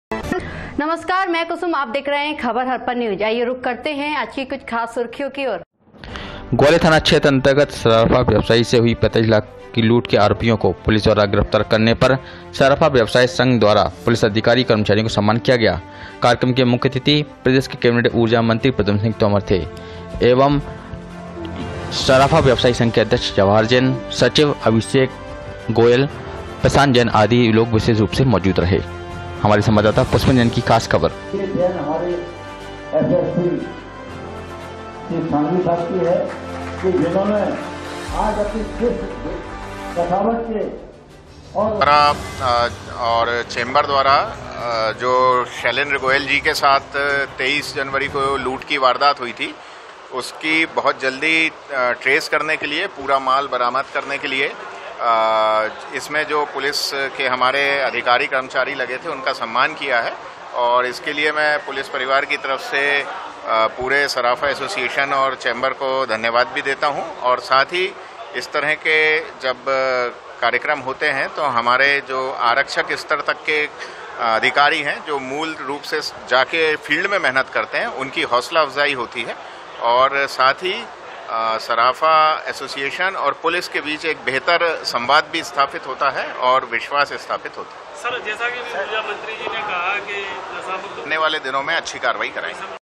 सर्राफा कारोबारी से हुई 45 लाख की लूट के आरोपियों को पकडने पर पुलिस कर्मियों का सम्मान
सर्राफा कारोबारी से हुई 45 लाख की लूट के आरोपियों को पकडने पर सर्राफा संघ ने किया पुलिस कर्मियों का सम्मान